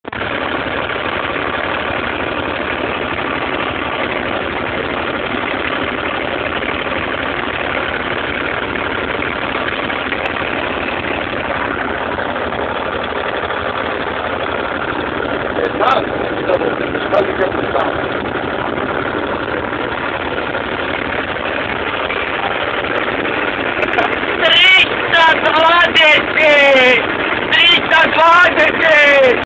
IMT Ferguson - pogresan smer
goda ga malo daje u levu stranu